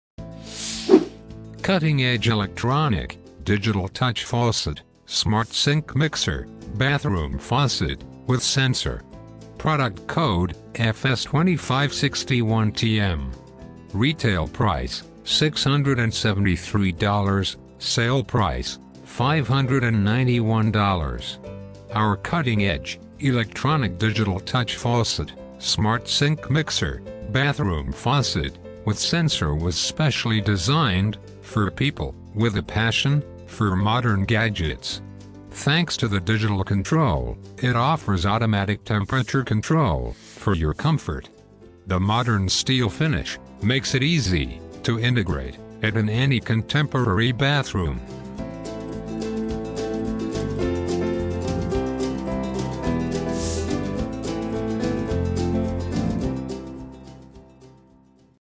Cutting Edge Electronic Digital Touch Faucet Smart Sink Mixer Bathroom Faucet with Sensor
Golden Colour Handles Dragon Sink Description :
Type - Digital Sink Faucet
Model Number - 2561TM
Brand Name - BathSelect
Type - Steel Ball Spool
Installation Type - Deck Mounted
Feature - Thermostatic Faucets
Valve Core Material - Brass
Faucet Mount - 4" Centerset